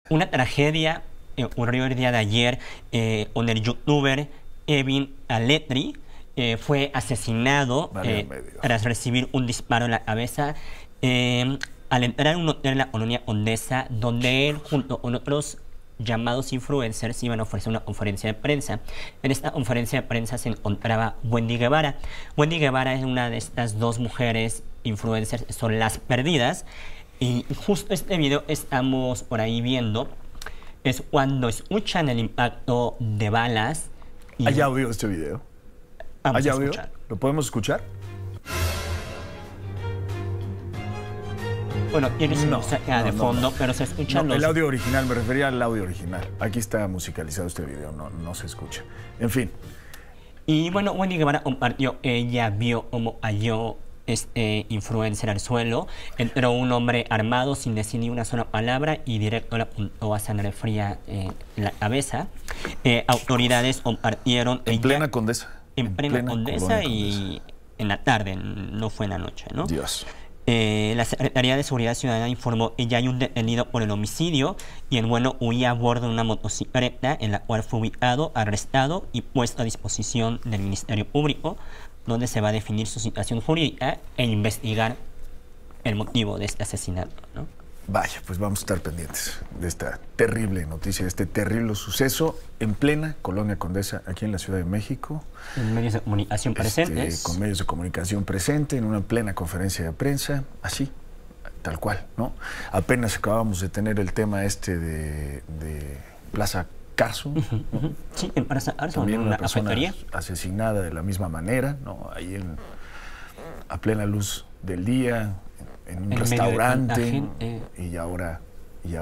El influencer Kevin Kaletry fue asesinado en la Condesa
El influencer Kevin Kaletry fue asesinado en la Condesa, al recibir un impacto de bala en la cabeza. Él y otros influencers se encontraban en el lugar para ofrecer una conferencia de prensa. Autoridades arrestaron al supuesto culpable por lo que se iniciarán las investigaciones pertinentes.

Grupo Fórmula

Canal sugerido

Descarga nuestra